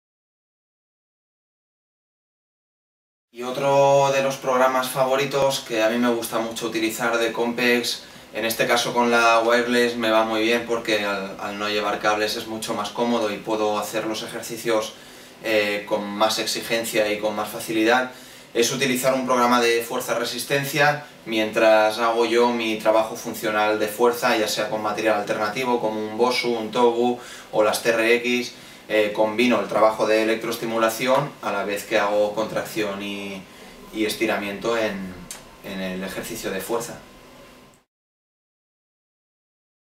Trucos Compex :Fuerza Resistencia por Jaume Leiva
Jaume Leiva, Campeón de España de Media Maratón 2012 ,  nos cuenta sus trucos sobre este programa